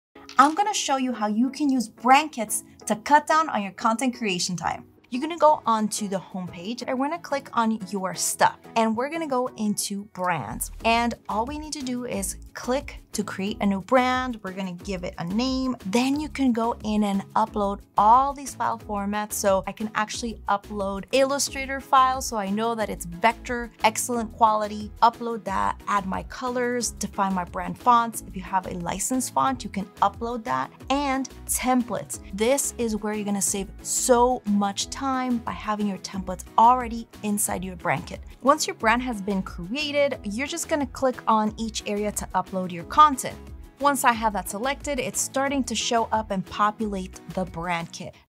How to Get Organized with Adobe Express Brand Kits
Ready to cut down on your content creation time?

It's time to utilize brand kits in Adobe Express!

Brand kits are an incredible way to organize your fonts, logos, brand assets, and more inside Adobe Express.

Plus, they help keep brand continuity front of mind for your entire team. Setting them up couldn't be easier, we can show you in less than a minute!